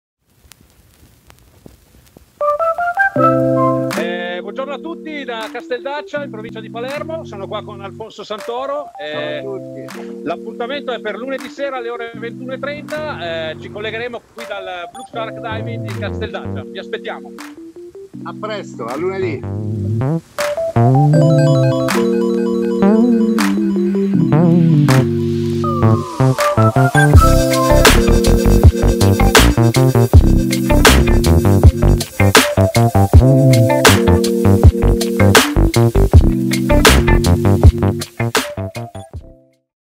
Premiere di "4 Chiacchere subacquee" Ep.13
La tredicesima puntata di "4 Chiacchere subacquee" in programma ogni lunedì alle 21,30 in diretta Facebook
Ideata e condotta da Gianluca Pomati
In collaborazione con Cmas Diving Center Italia - Cmas Divers Community